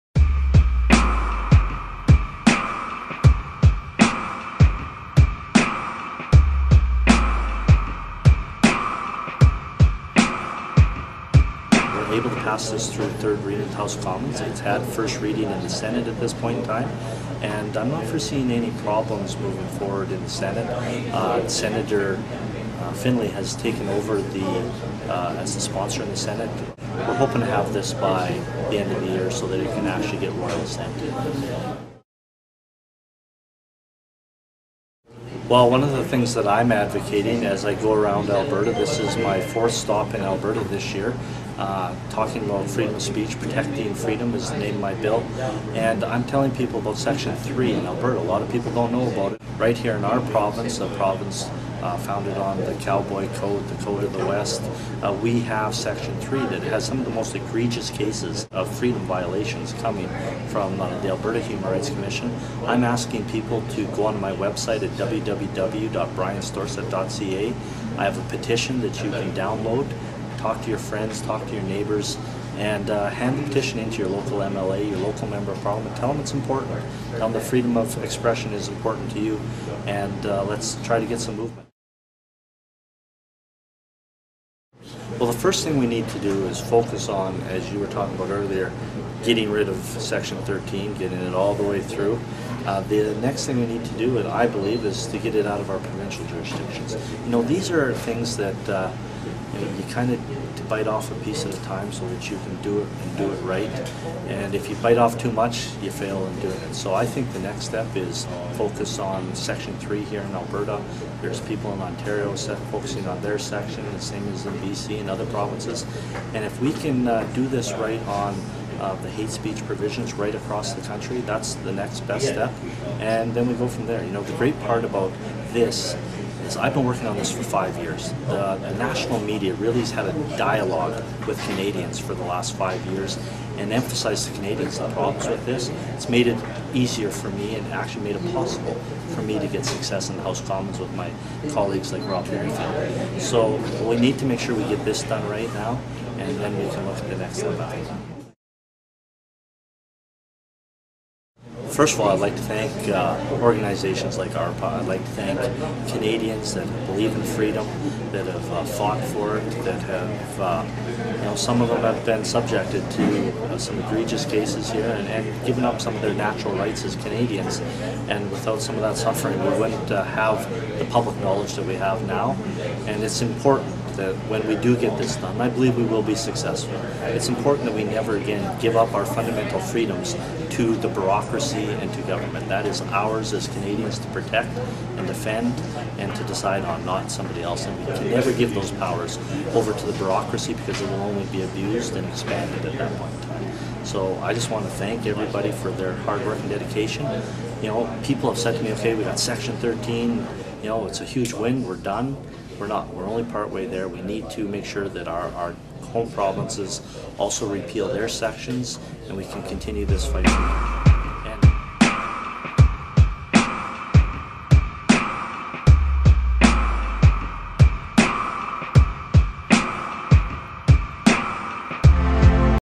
Brian Storseth interview with Barrhead/Neerlandia ARPA
The Barrhead Neerlandia ARPA group hosted an info evening with Member of Parliament Brian Storseth on the topic of freedom of speech and the Human Rights Commissions. This is an interview with Mr. Storseth recorded that evening.